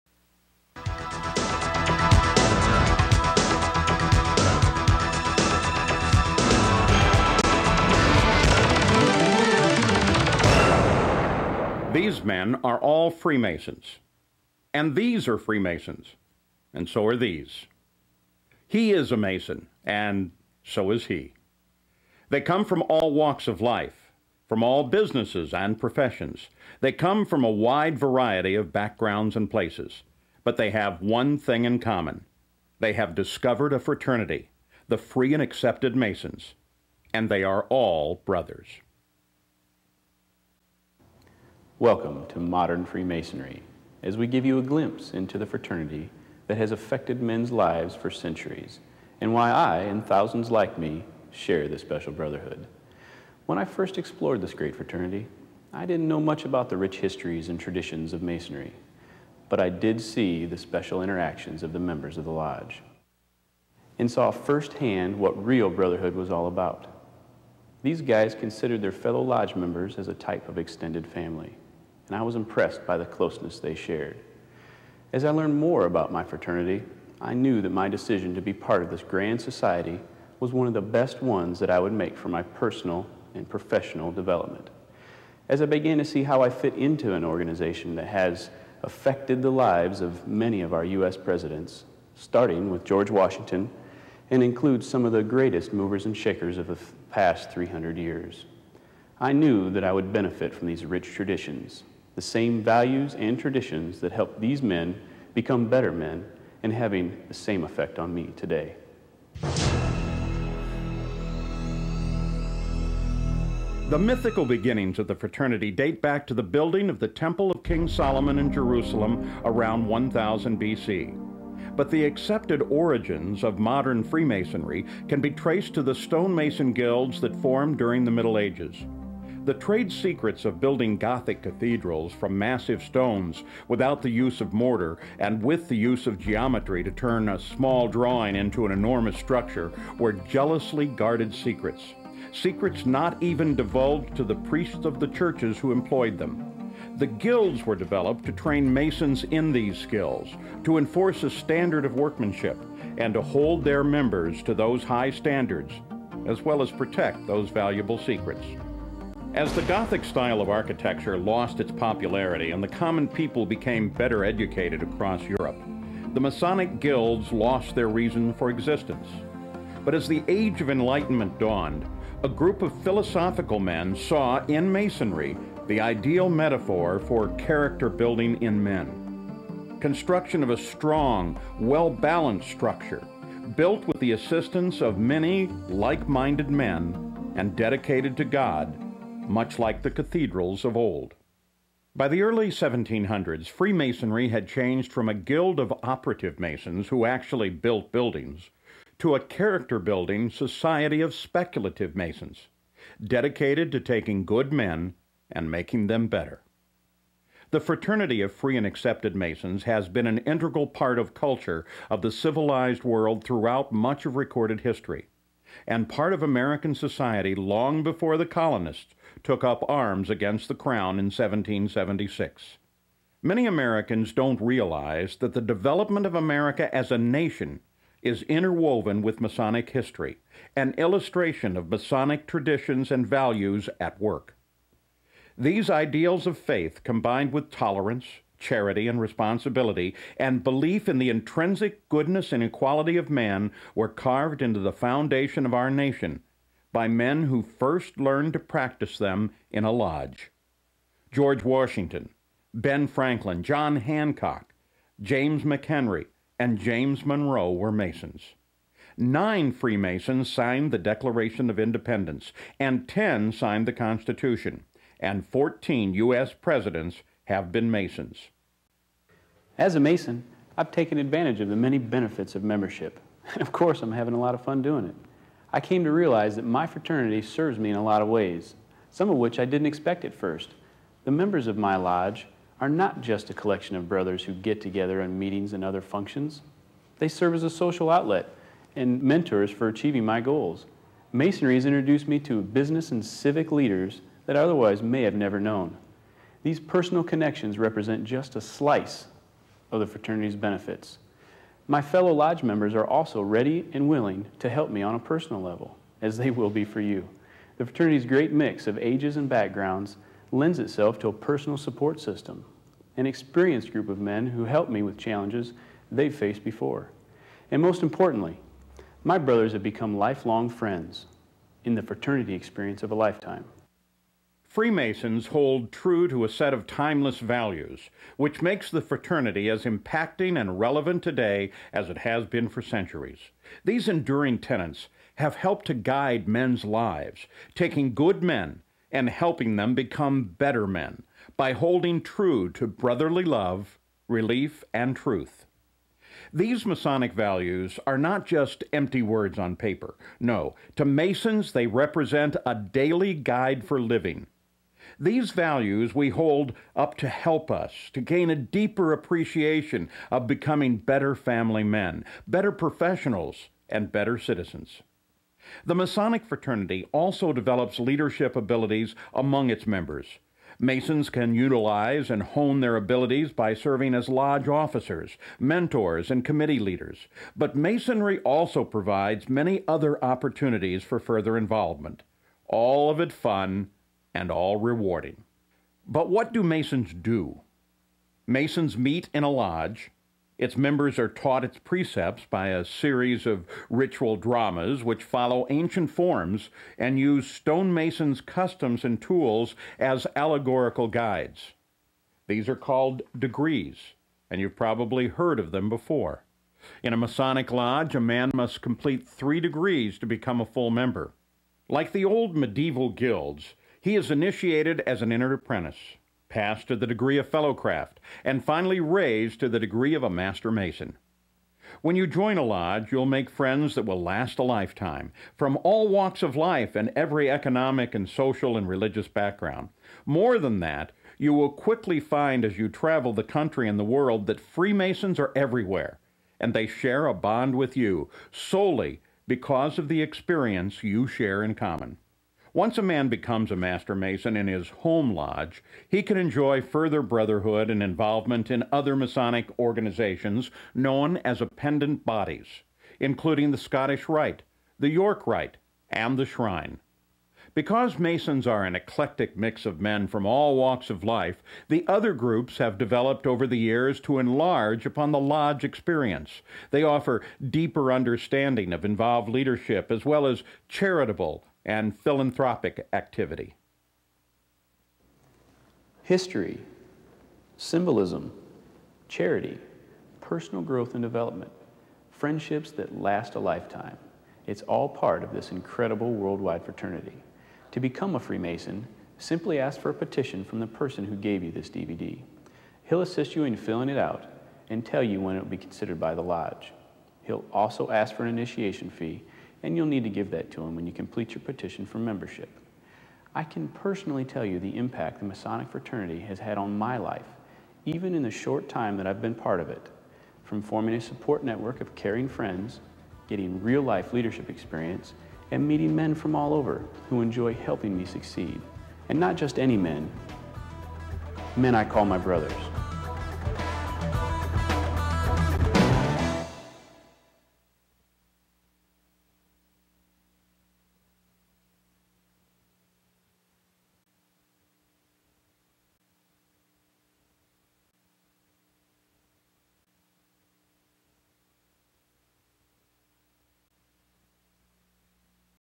What Is Freemasonry
Freemasonry is the world's oldest fraternity, based on symbolism with a belief in God at its core.  Freemasonry is not a secret organization; it is an organization with secrets.  Freemasonry makes good men better.